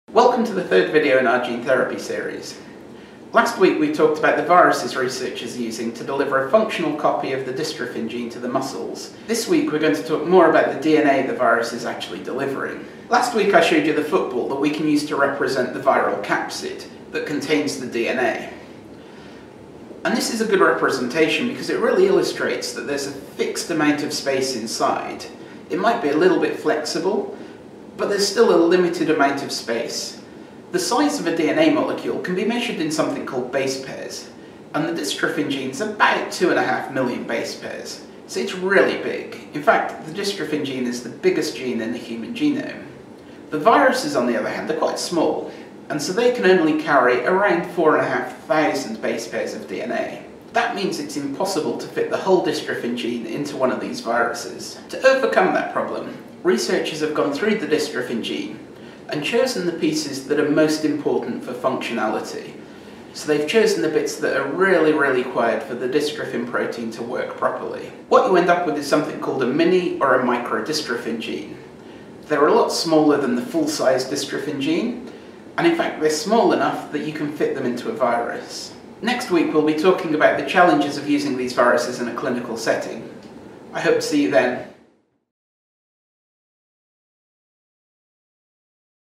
Duchenne Research Explained: Gene Therapy Series, Episode 3
We are pleased to present the third video in our Duchenne research series on gene therapy.

This week we find out how researchers developed mini and micro dystrophin genes that can be carried to the muscles by AAV viruses.

Let us know if you have any questions - at the end of the series we will be hosting a follow up Q&A episode!